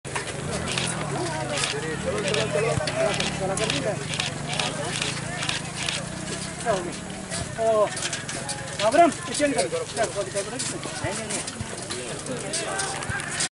13 Tripura BN NCC held Fit India Bicycle rally
13 Tripura BN NCC held Fit India Bicycle rally in Agartala. TIWN Video Sep 11, 2020